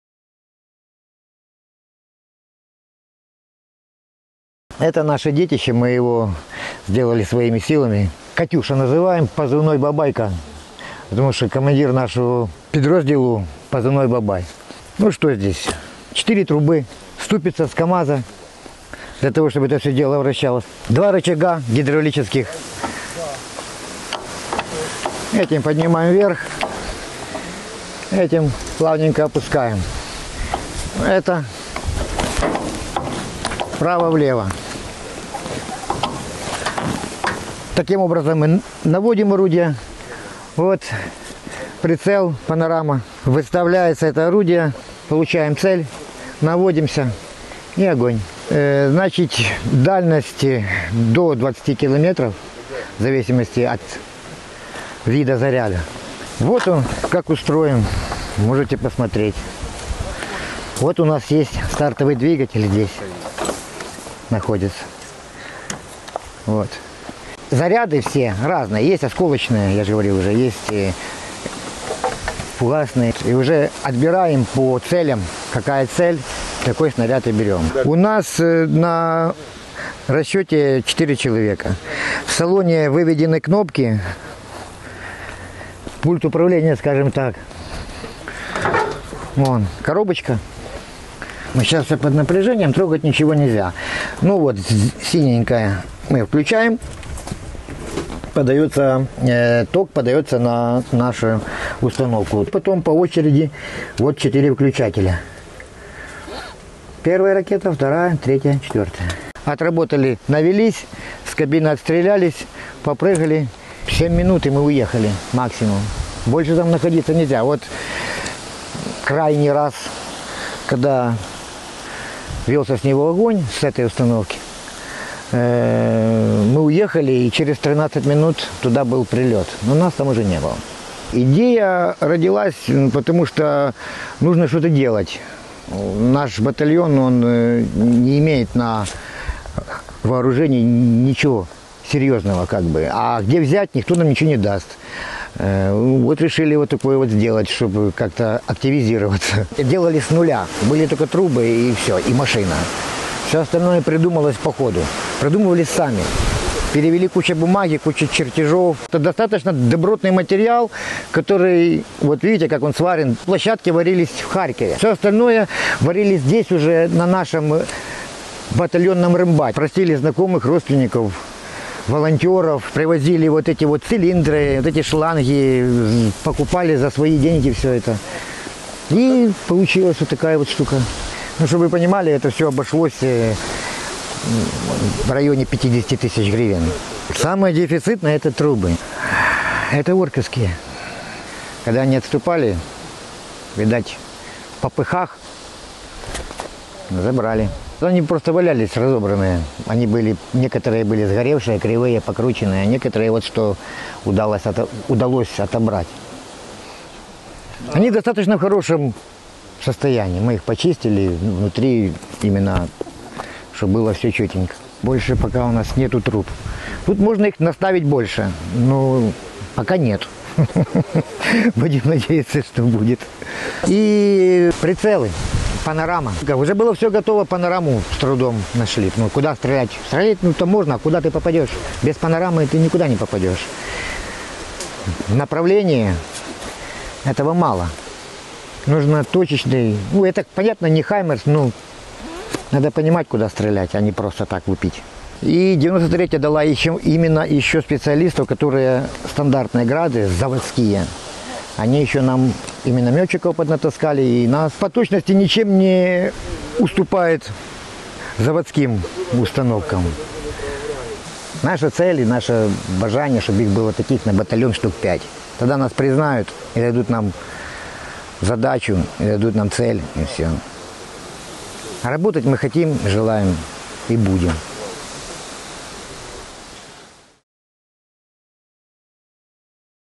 «Бабайка» з ТрО знищує росіян. Як самостійно зробити установку «Град»?
Репортаж зі 113-ї бригади ТрО, яка боронить Харьківщину

Джерело: пресслужба Сил Територіальної Оборони